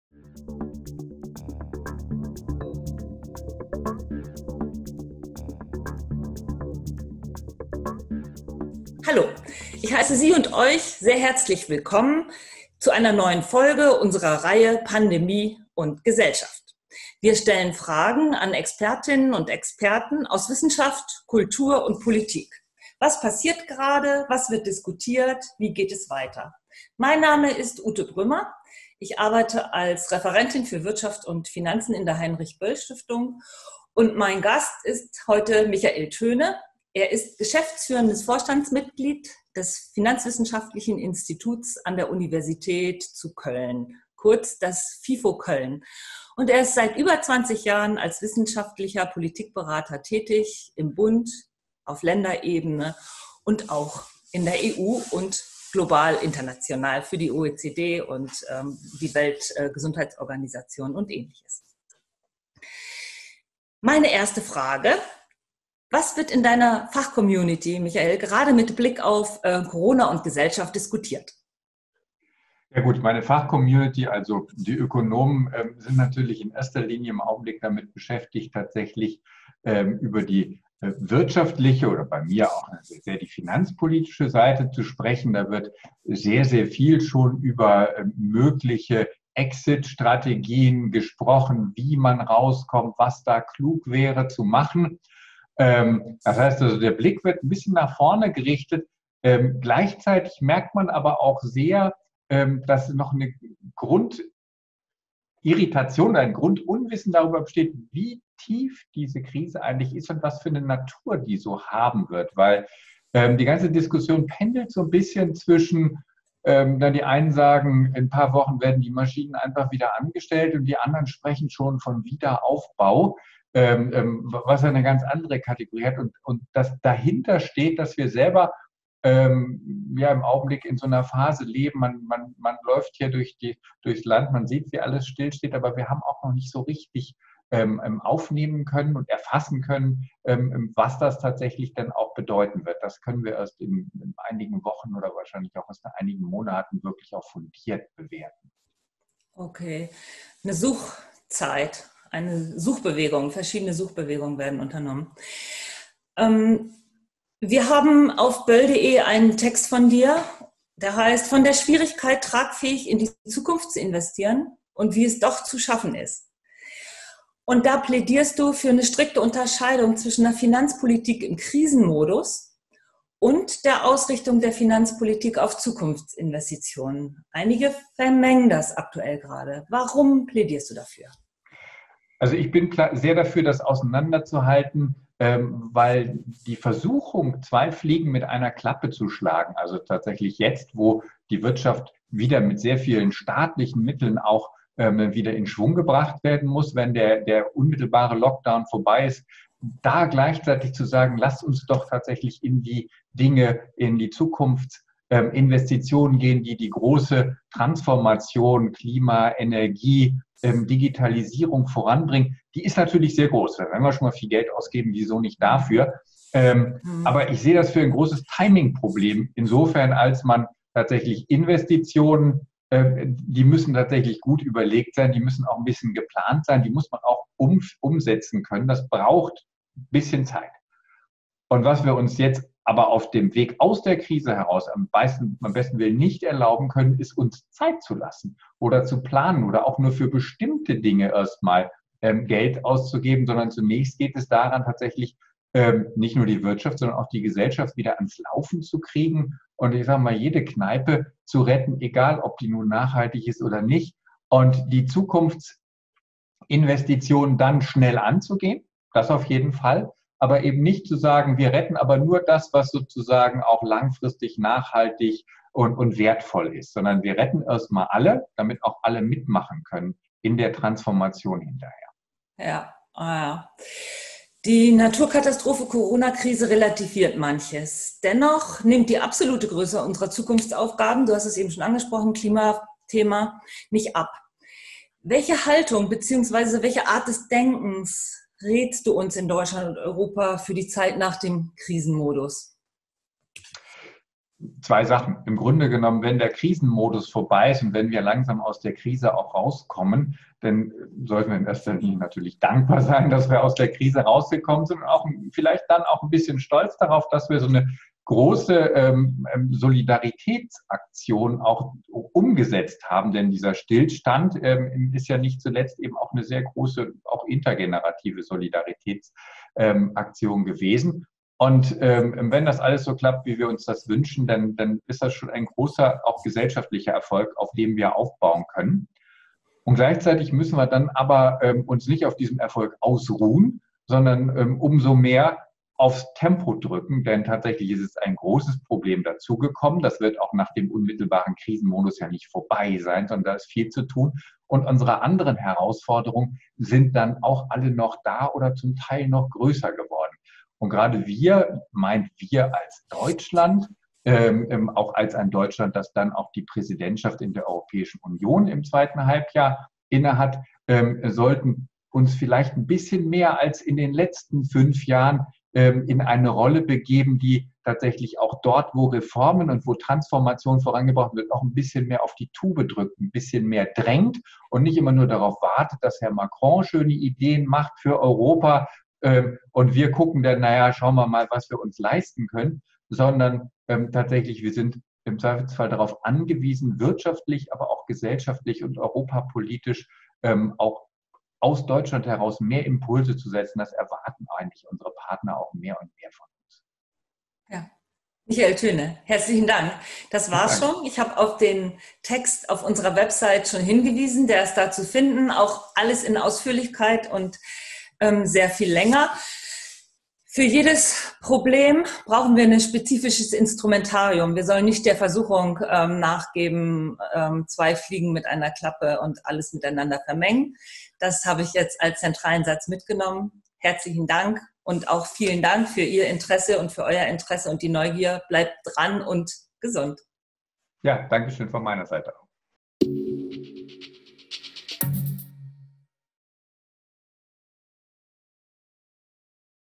Pandemie & Gesellschaft #6 - Tragfähige Zukunftsinvestitionen in Zeiten der Corona-Pandemie
Es gilt, die jetzt akuten Herausforderungen von Corona und Konjunktur und die längerfristigen Zukunftsaufgaben in der Gesamtbetrachtung auseinanderzuhalten, um beide Dimensionen angemessen ernst zu nehmen.

Ute Brümmer, Referentin Wirtschaft und Finanzen, Heinrich-Böll-Stiftung, spricht mit Michael Thöne vom Finanzwissenschaftlichen Forschungsinstitut an der Universität zu Köln.

Zum Weiterlesen:
Kommentar von Michael Thöne: "Zukunftsinvestitionen in Zeiten der Corona‐Pandemie"